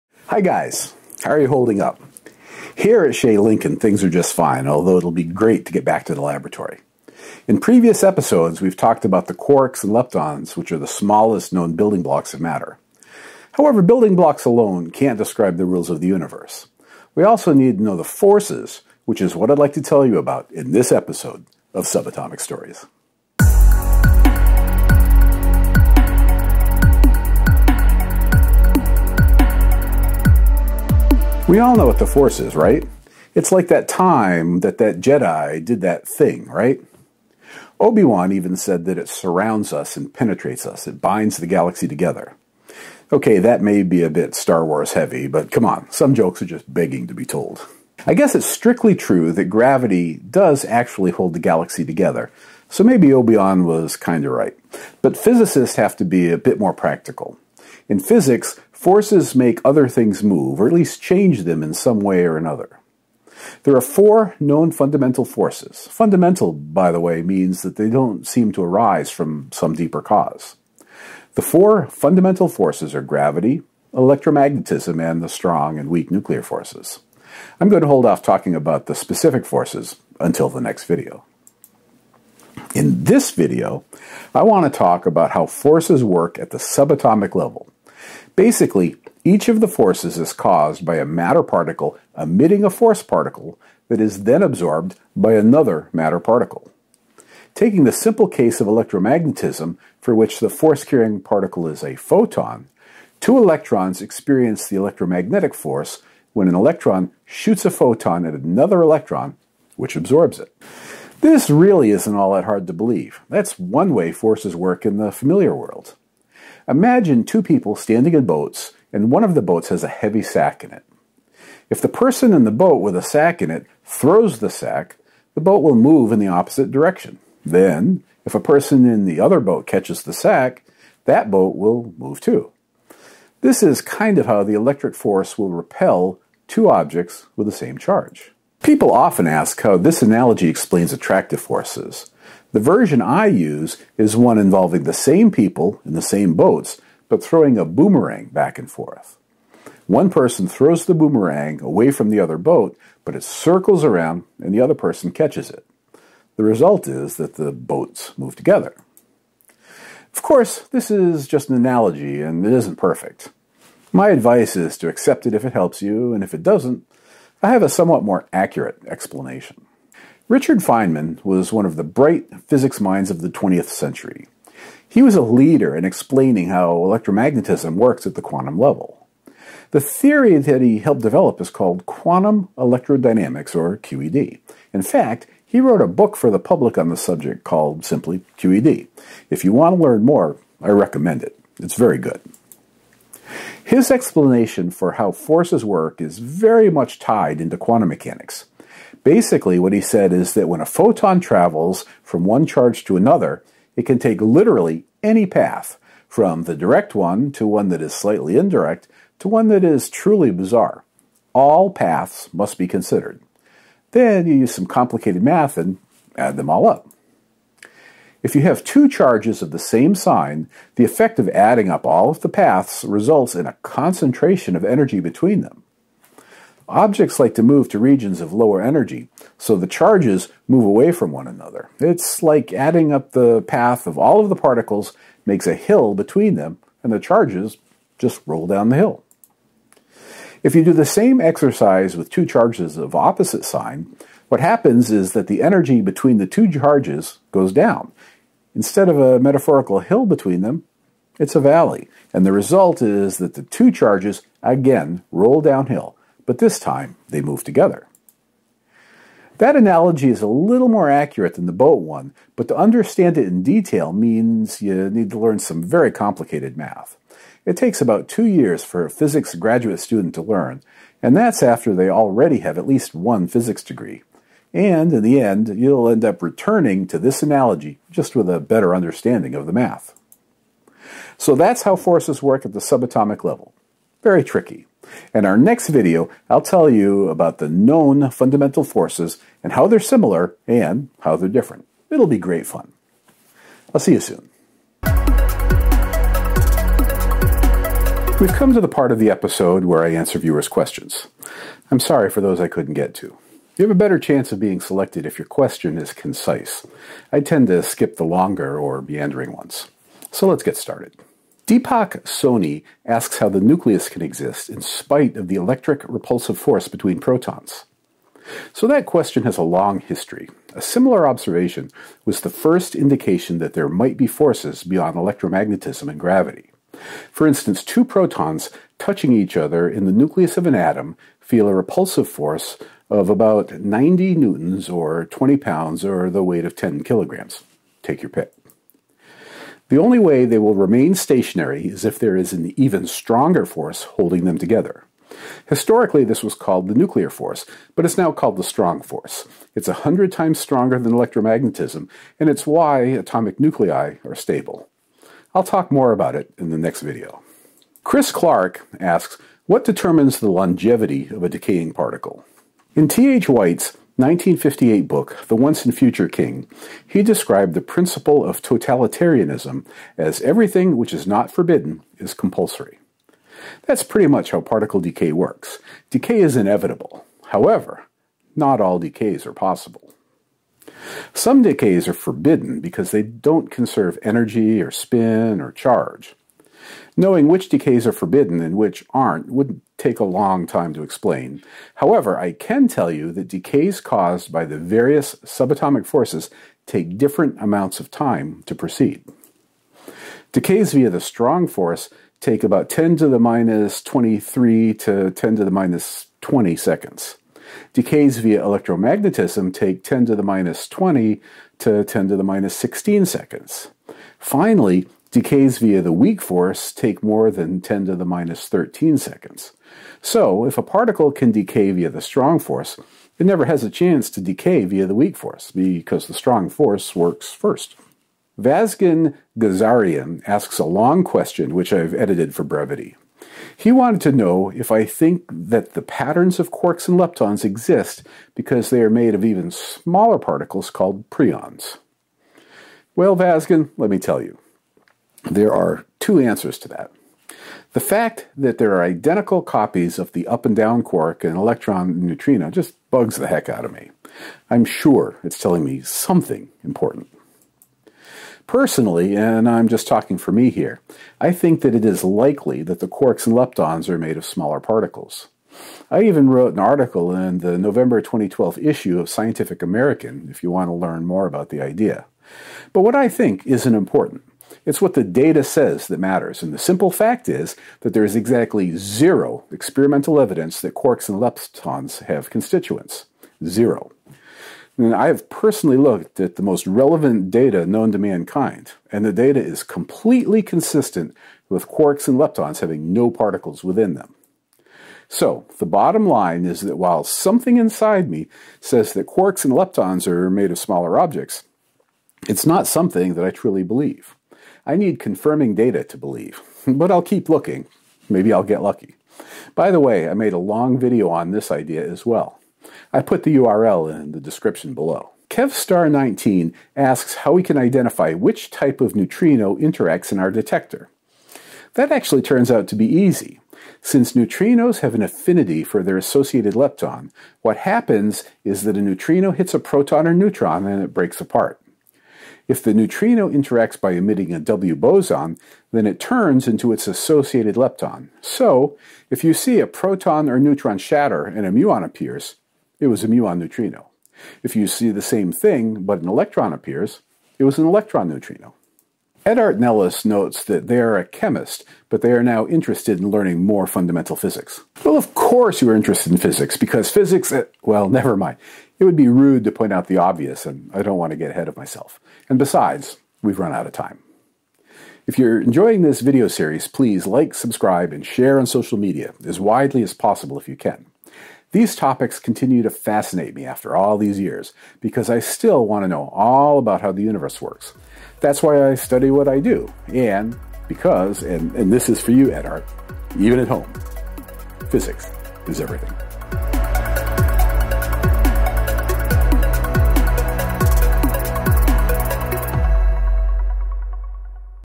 5 Subatomic Stories: Forces the Feynman way
Subatomic forces at the quantum level are best understood as a cloud of force-carrying particles jumping from one object to another. In Episode 5 of Subatomic Stories, Fermilab’s Dr. Don Lincoln gives a brief explanation of this phenomenon, including two analogies for how this complicated mathematics can be understood.